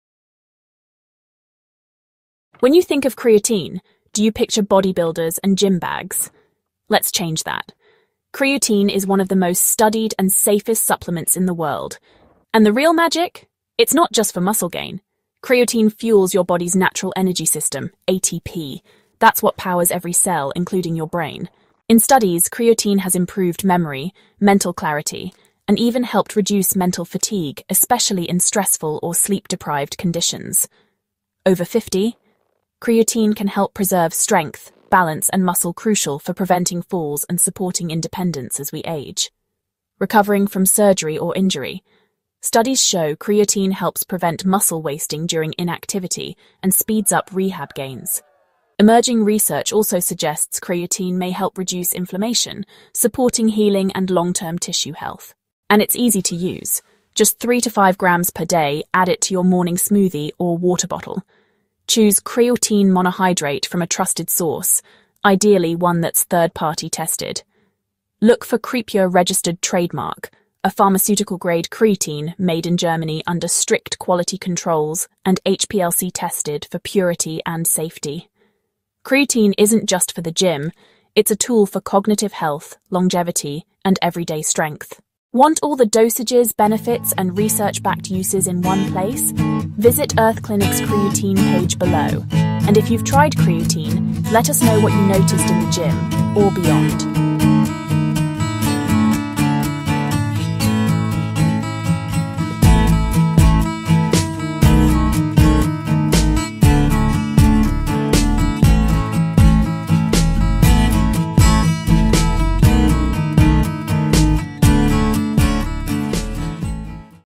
Creatine Benefits Beyond Muscle: Brain, Recovery & Aging - Earth Clinic
Discover the full-body benefits of creatine beyond muscle building. In this video, we explore the latest research and real-world uses for this powerful, natural compound.

✅ Supports muscle growth, strength, and performance
✅ Aids in post-workout recovery and reduces soreness
✅ Boosts memory, mental clarity, and cognitive endurance
✅ May reduce inflammation and support healing
✅ Protects brain and nerve cells—studied for Parkinson’s, ALS & more
✅ Helps preserve muscle mass and function after age 50
✅ Speeds up recovery during periods of injury or surgery
✅ Improves insulin sensitivity and glucose metabolism
✅ Safe daily dosages for active and sedentary lifestyles
✅ What to look for in a high-quality creatine (Creapure® explained)
✅ Tips for adding creatine to your daily rountine

Research Creatine products on Amazon